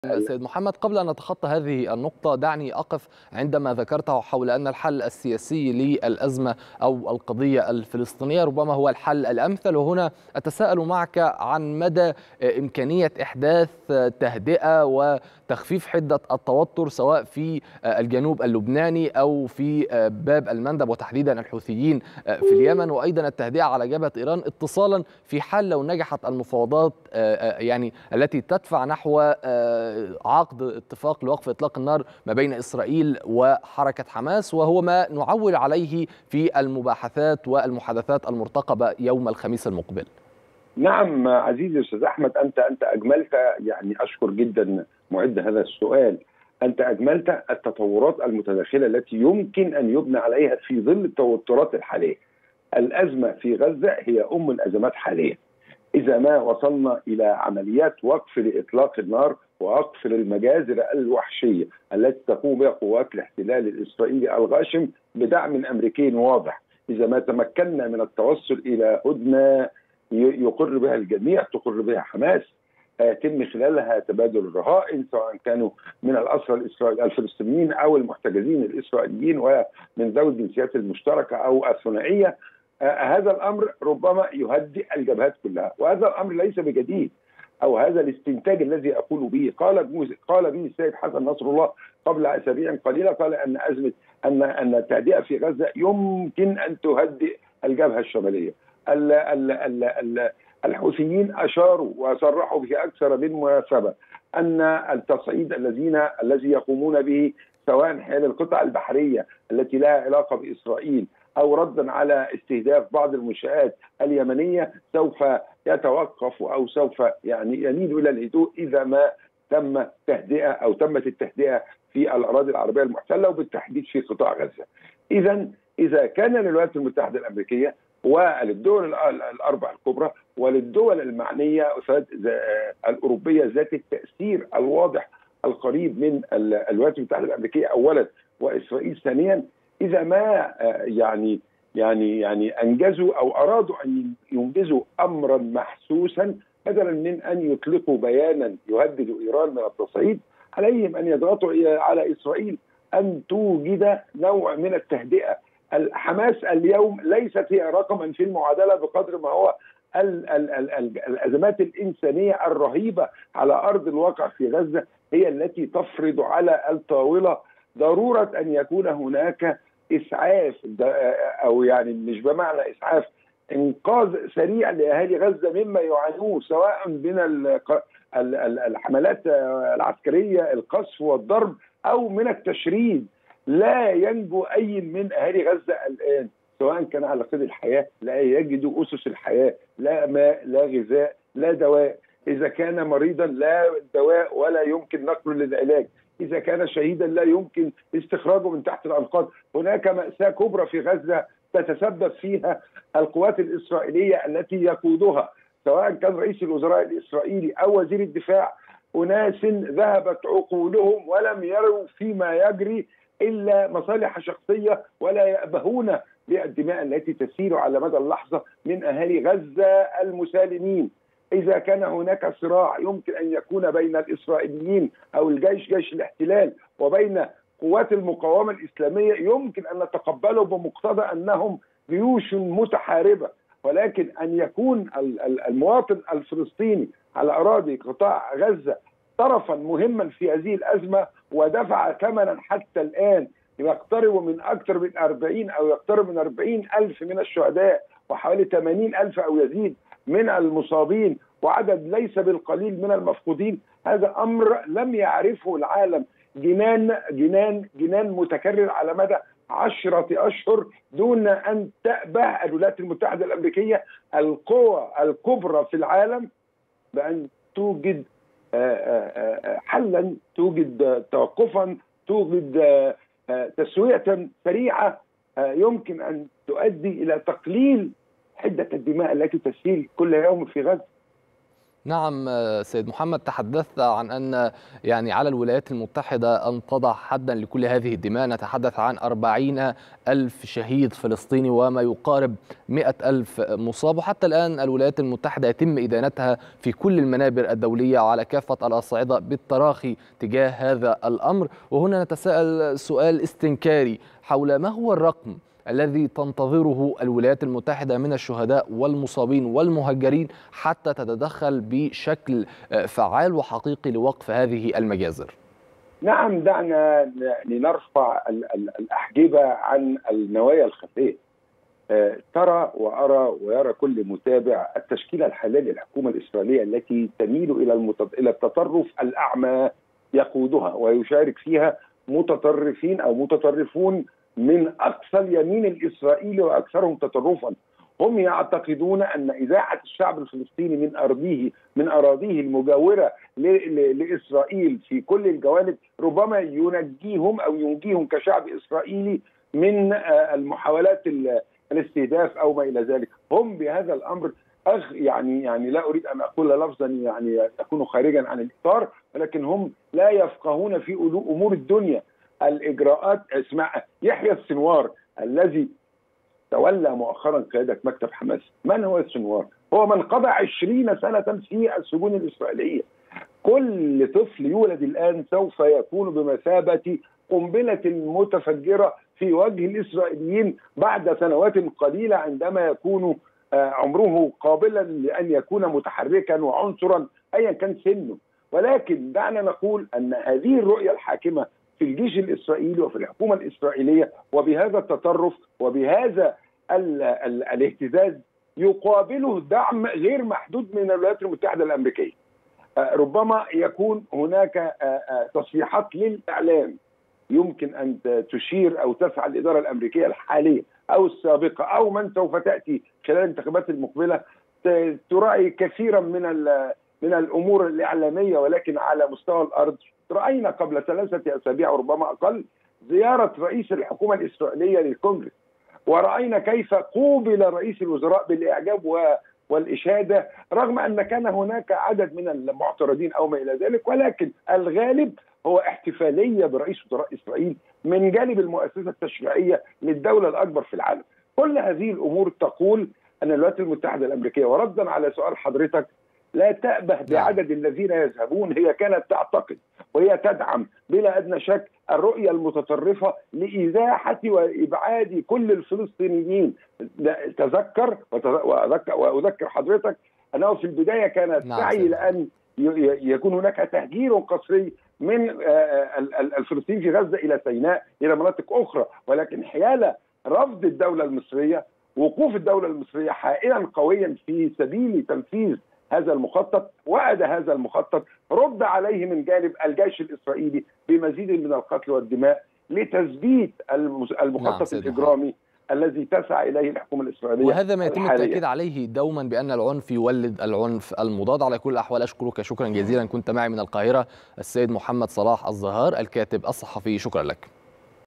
الصحفي محمد صلاح الزهار: التوصل إلى تهدئة في غزة يمكن أن يؤدي إلى تهدئة جميع الجبهات
الكاتب الصحفي محمد صلاح الزهار: التوصل إلى تهدئة في غزة يمكن أن يؤدي إلى تهدئة جميع الجبهات